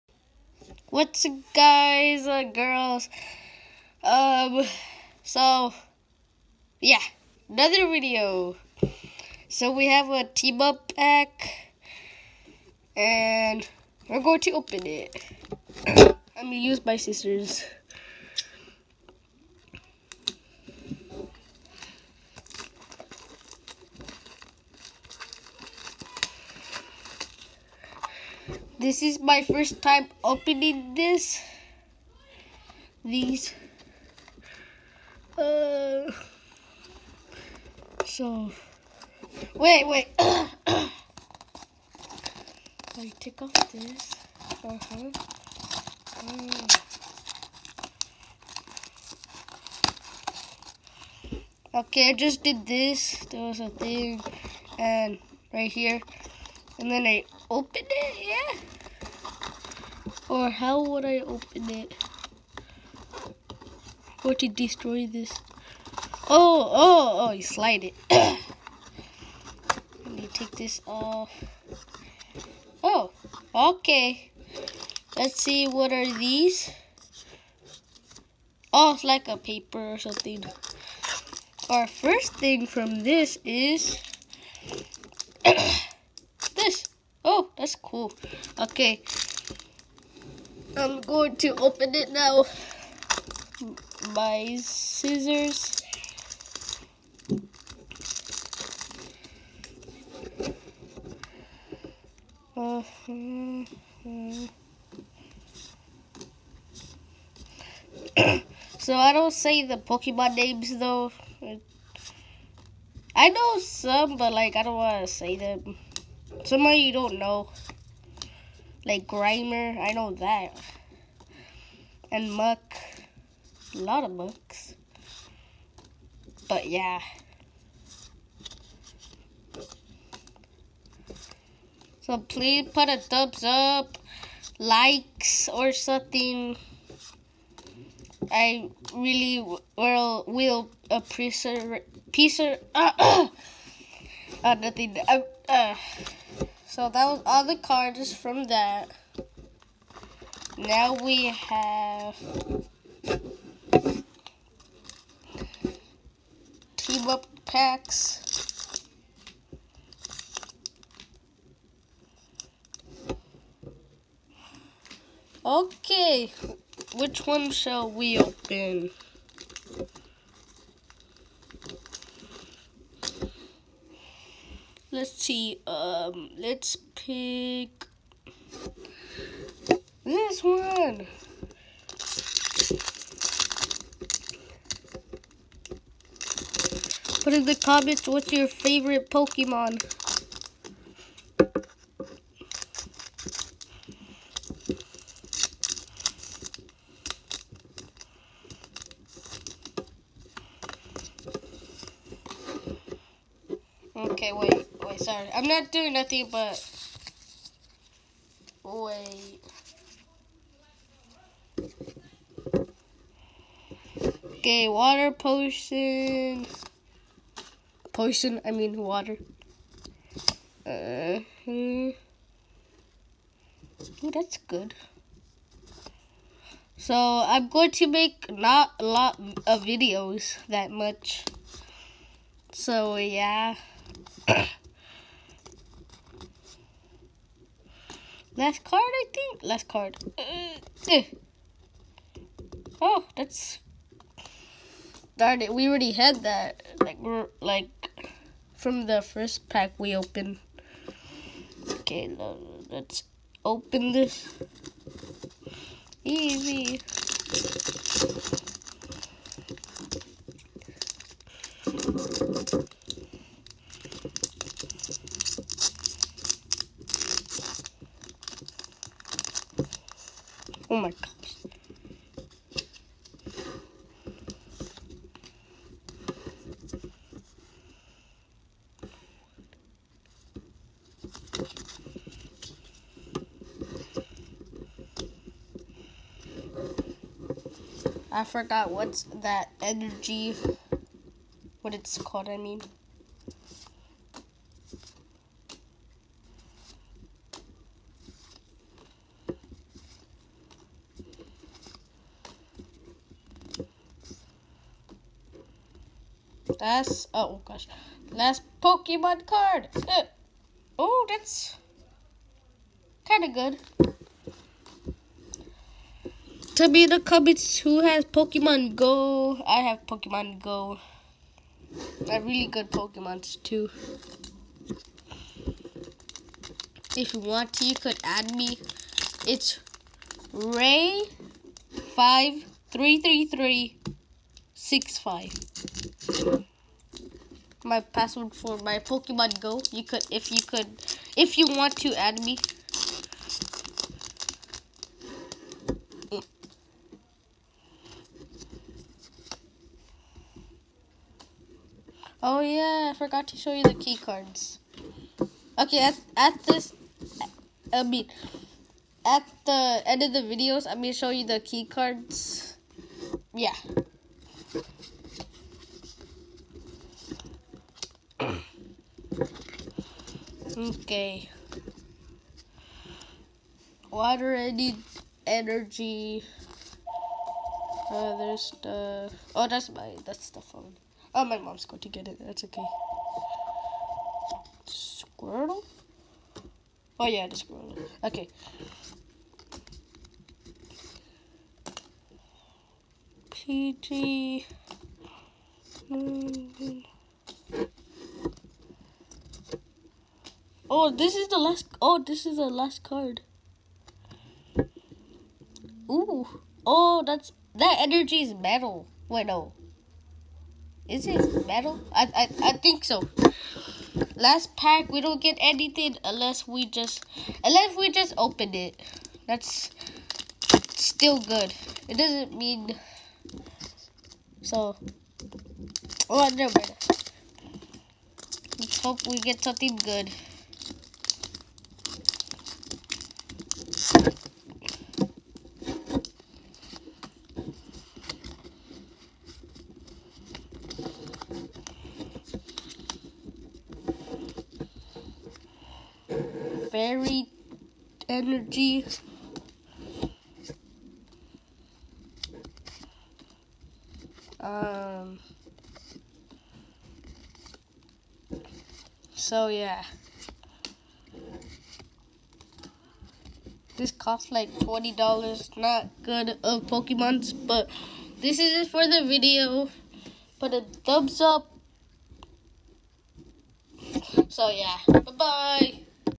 Opening a Team up Box
Opening a team up Box But I forgot something I will do it less video The Chi codes